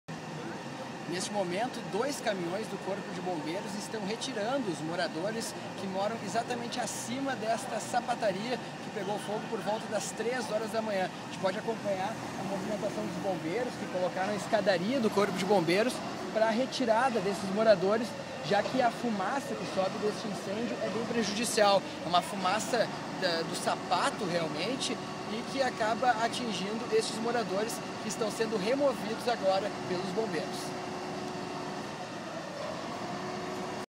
Incêndio atinge prédio na Protásio Alves
Durante madrugada um incêndio atingiu um prédio na Protásio Alves. O repórter Voltaire Santos acompanhou. Veja o vídeo.